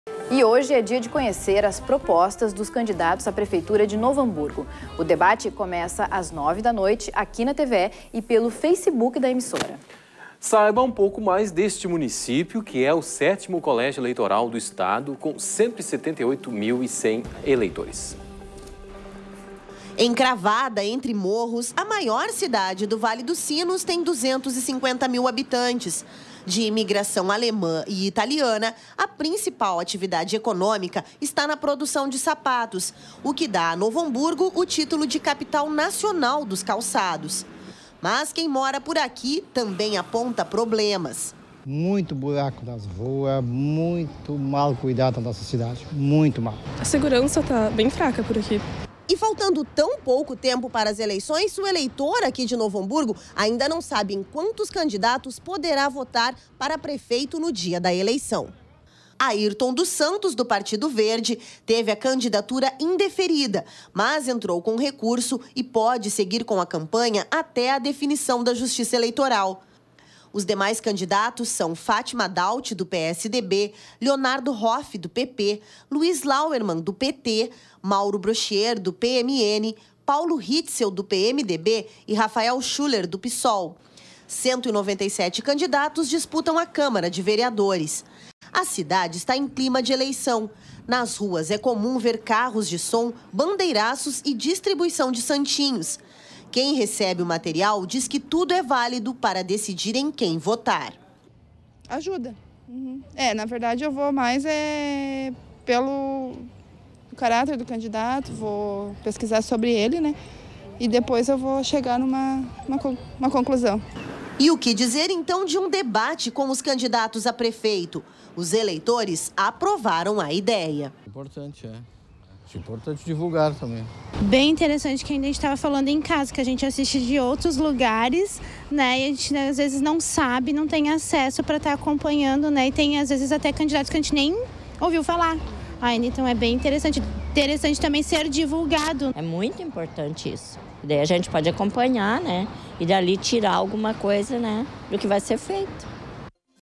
Hoje tem DebateNaTVE Novo Hamburgo
Hoje é dia de conhecer as propostas dos candidatos à prefeitura de Novo Hamburgo. O debate começa às 21h, na TVE e pelo faceboob/tvepublicars. Saiba um pouco mais sobre o município, que é o 7º colégio eleitoral do Estado, com 178,1 mil eleitores.